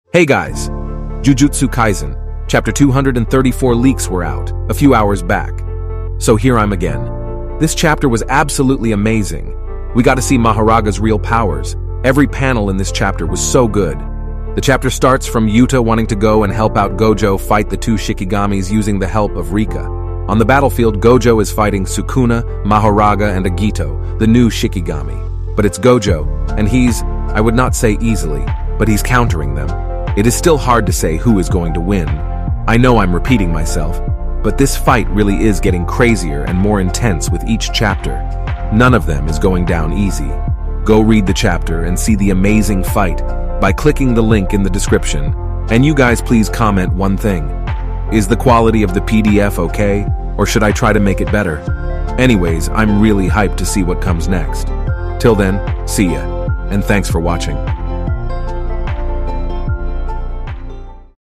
Mahoraga's true power!! Jujutsu Kaisen chapter 234 LEAKS!
Hey guys. Jujutsu Kaisen chapter 234 leaks have started to come out. So, I decided to make a video telling you guys about it. Hope you'll like it.

Arigato Gozaimasu! Jya matta...
[Thank you ! Until we meet again. ]